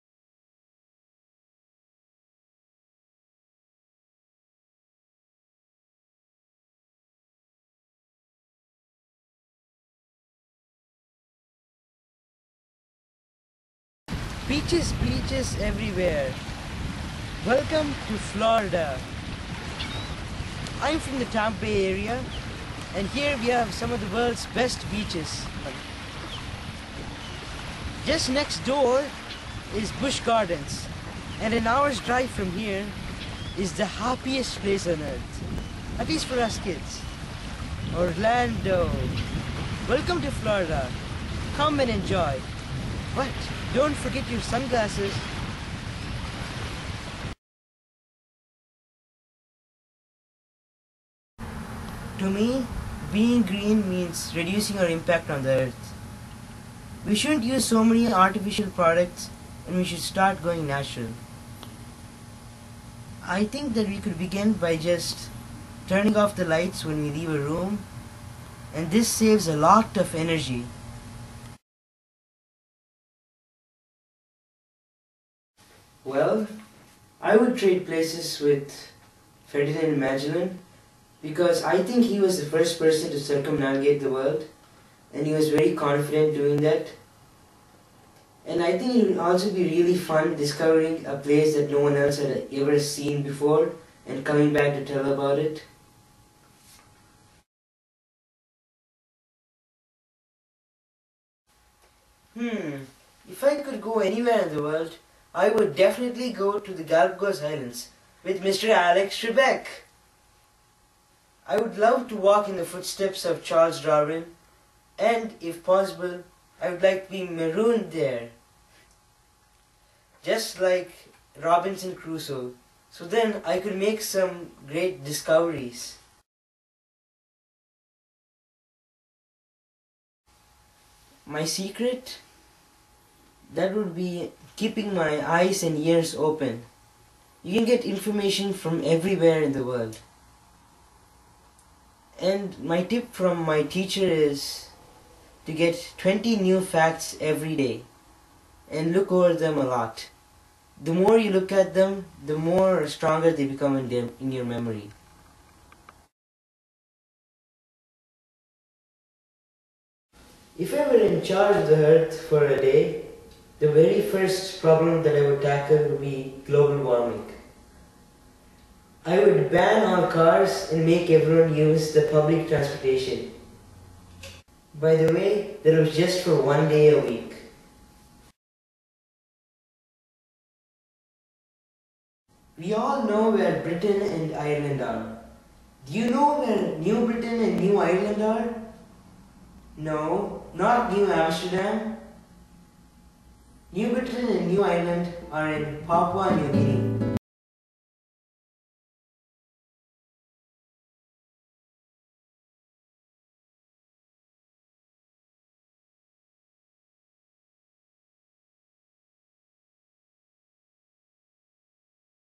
Geographic Bee 2010 - FL Finalist | National Geographic Bee 2010
Aadith will be competing at the National Geographic Bee championship in Washington, D.C.